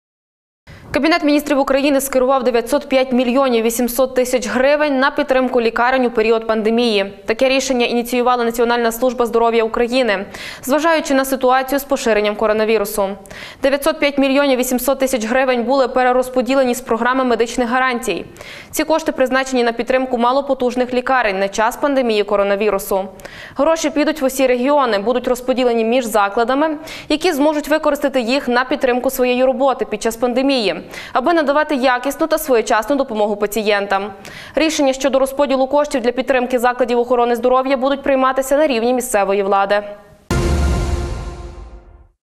Підтримка лікарень
Кабінет Міністрів скерував 905, 8 млн. грн. на підтримку лікарень у період пандемії. Таке рішення ініціювала Національна Служба здоров`я, зважаючи на ситуацію з поширенням коронавірусу. 905,8 млн грн перерозподілять з Програми медичних гарантій. Ці кошти призначені на підтримку малопотужних лікарень на час пандемії коронавірусу. Гроші підуть в усі регіони, будуть розподілені між закладами, які зможуть використати їх на підтримку своєї роботи під час пандемії, аби надавати якісну та своєчасну допомогу пацієнтам. Рішення щодо розподілу коштів для підтримки закладів охорони здоров’я будуть прийматися на рівні місцевої влади.